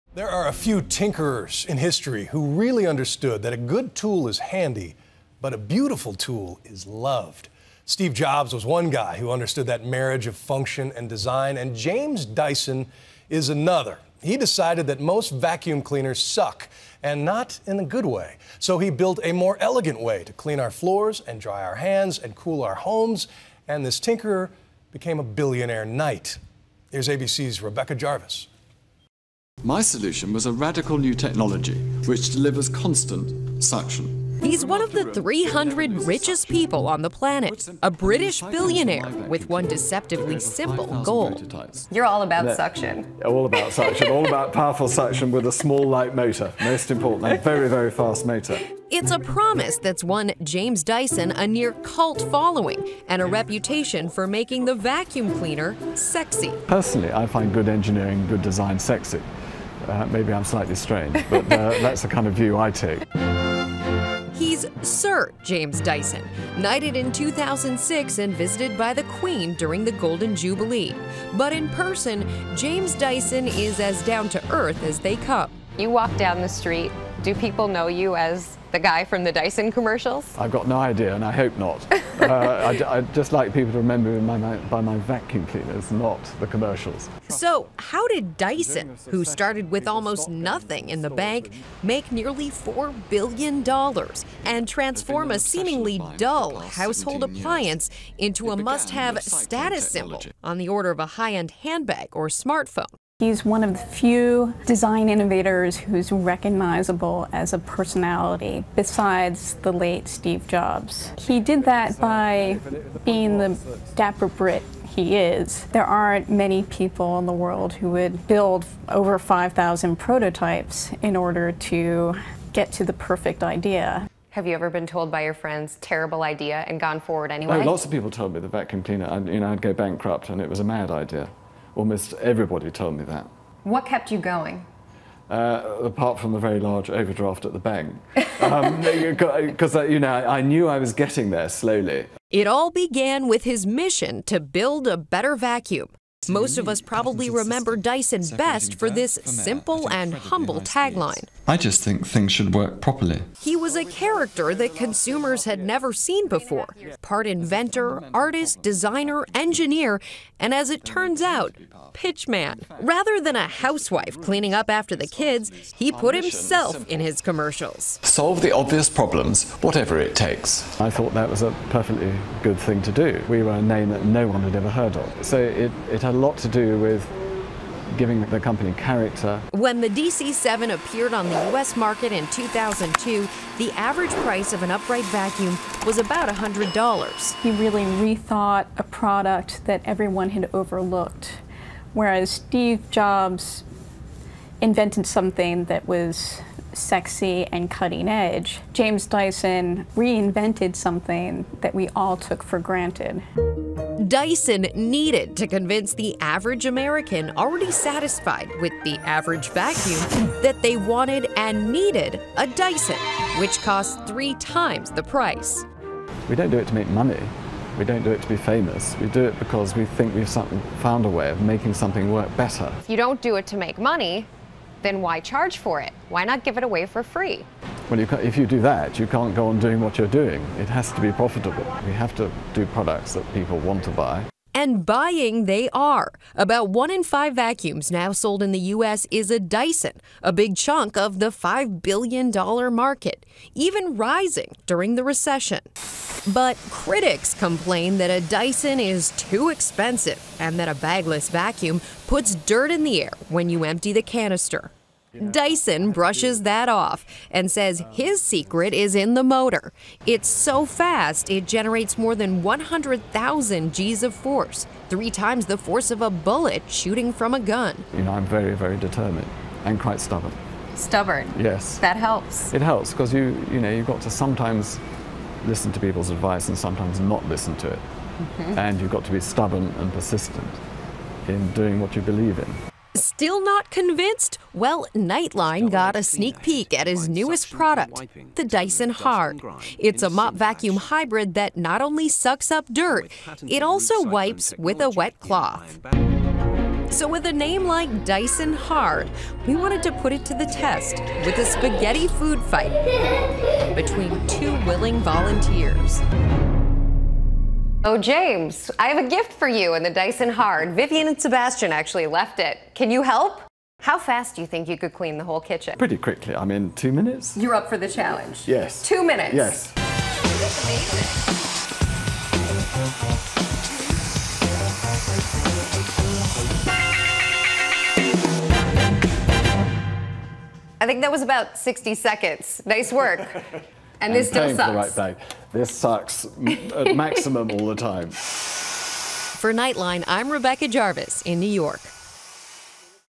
James Dyson, Making Homemaking Sexy
The self-made man whose empire is worth billions revolutionized the vacuum cleaner.